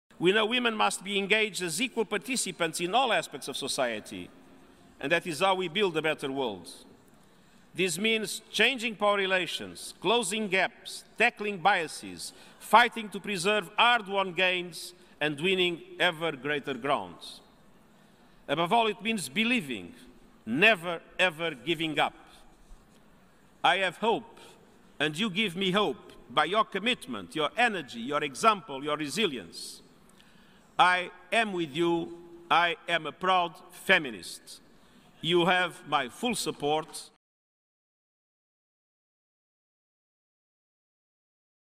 FN:s generalsekreterare: "Jag är en stolt feminist"
Antonio Guterres varnade på måndagen att kvinnors rättigheter är på tillbakagång. I ett tal till FN kallade han sig "stolt feminist" och sa att kampen för jämställdhet måste vinnas.

Bilder AP/FN Antonio Guterres på Twitter